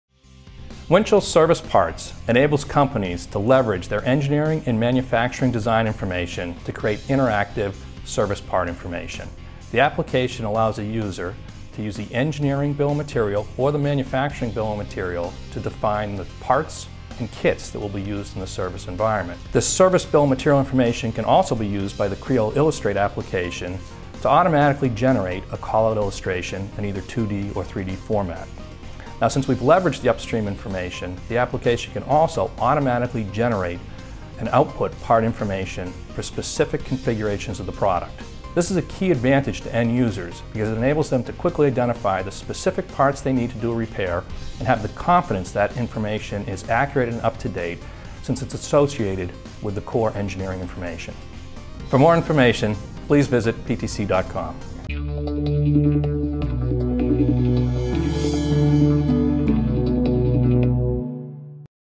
Windchill Service Parts - PTC
Tom Sears, Senior Director Product Management, introduces Windchill Service Parts, which enables the creation, management, and delivery of spare parts information.